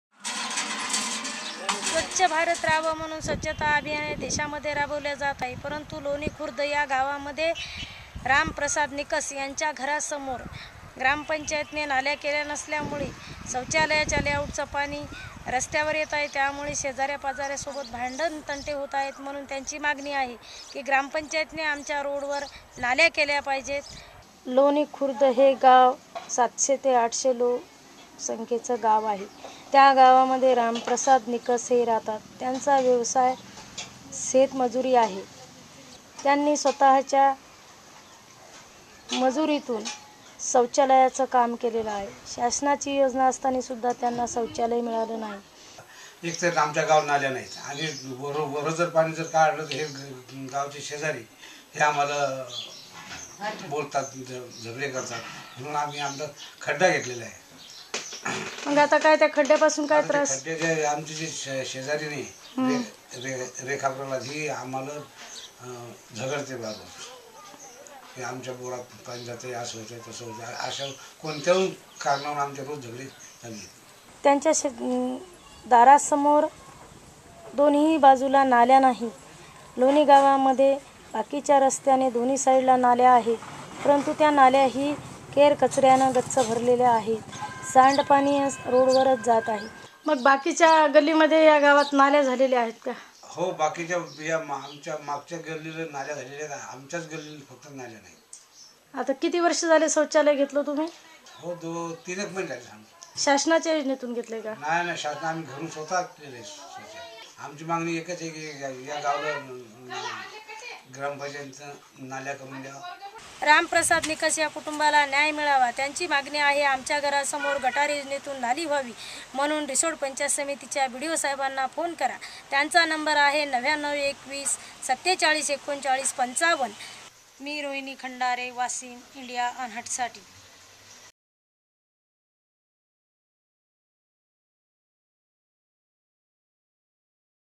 Sewage water runs on roads in Lonikhurd, Maharashtra
Rohini Khandare reports from Lonikhurd, Maharashtra for Video Volunteers.